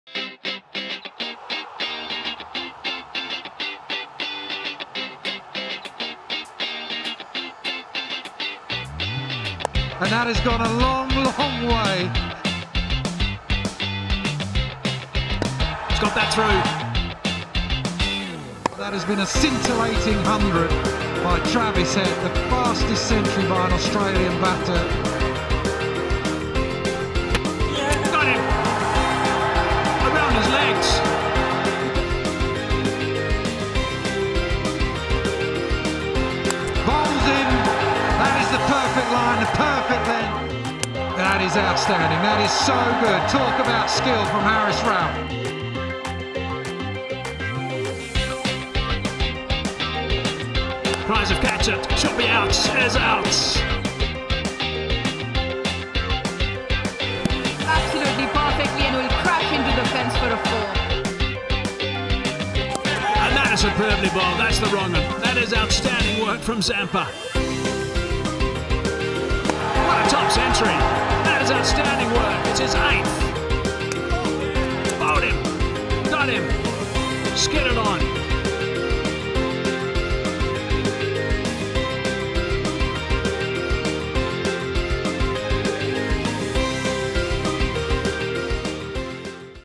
Closer | Pakistan vs Australia | 1st ODI 2022 | PCB | MM2T
The visitors continue their winning streak in Lahore.